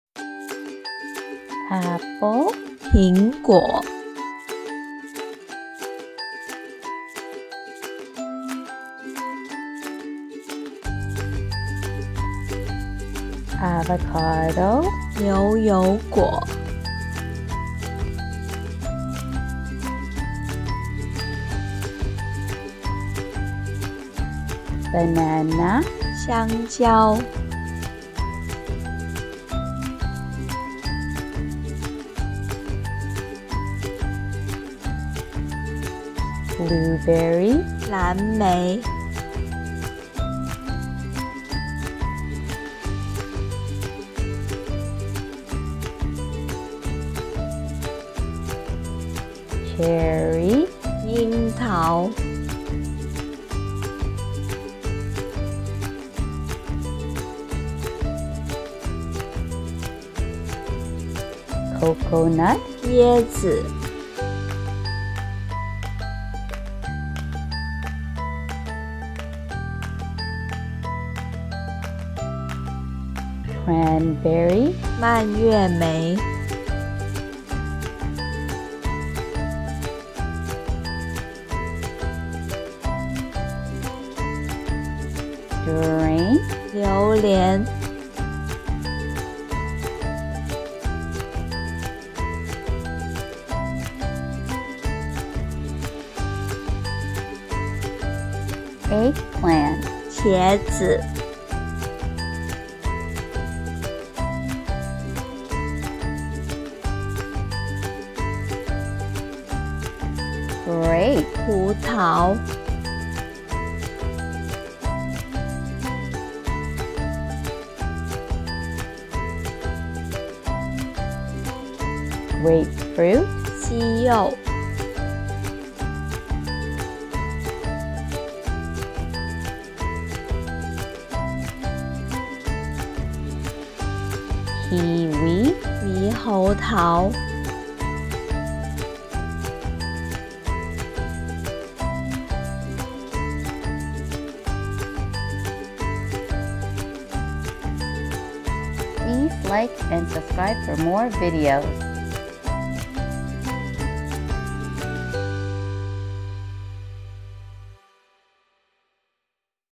Learning Fruits 学习水果 English and Mandarin for babies and kids Fruit Vocabulary
学习水果
Here's a fun and easy way for your babies, toddlers and kids to learn the vocabulary of many varieties of fruits.  This video will have the speaking and spelling of the fruits for extra learning materials
There will be more educational videos like this in different languages too.